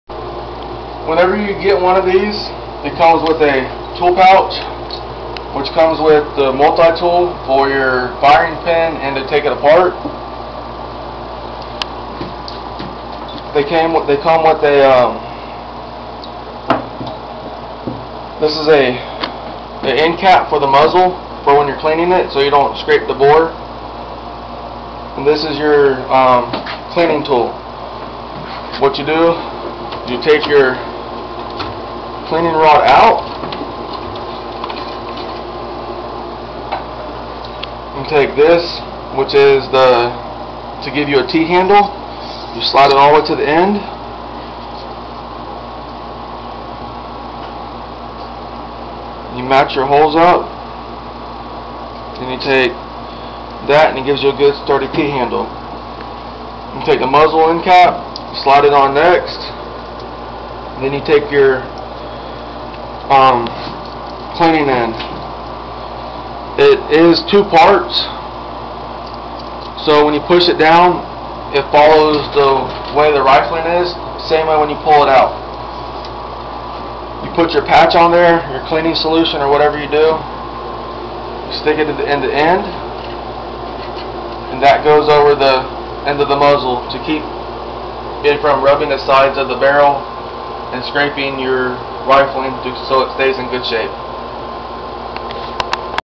tool bag
showing how to use the clean tools that come with it